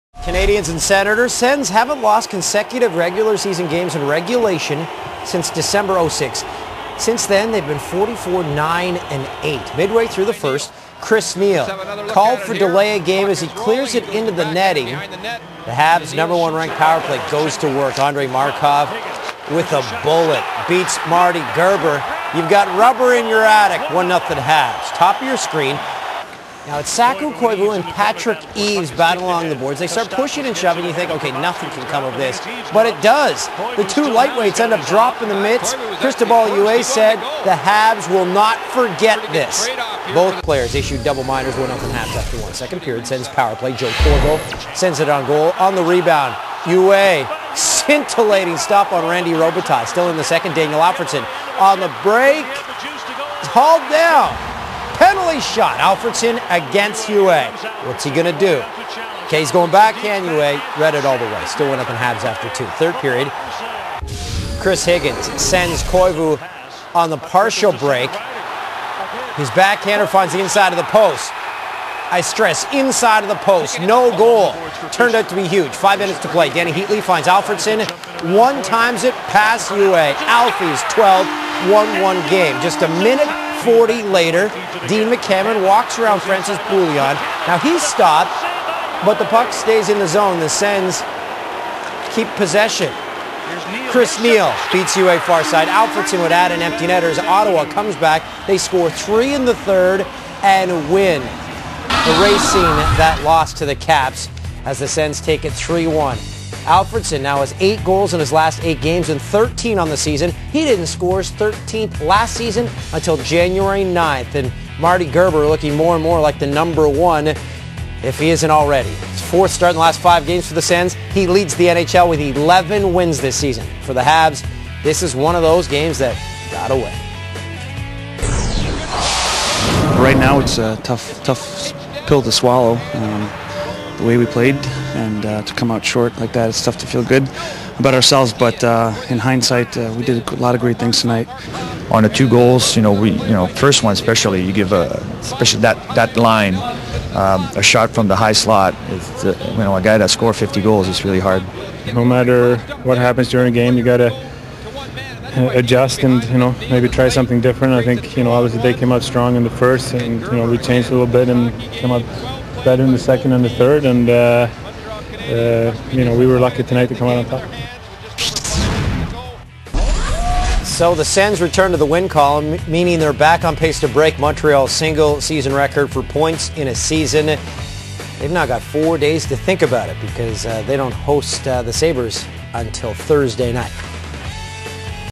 NHL: Montreal Canadiens @ Ottawa Senators (Nov. 10/07)
Highlights: MTL 1, OTT 3
Chris Neil scored the game-winner and Daniel Alfredsson had a pair as the Sens rallied to beat the Canadiens.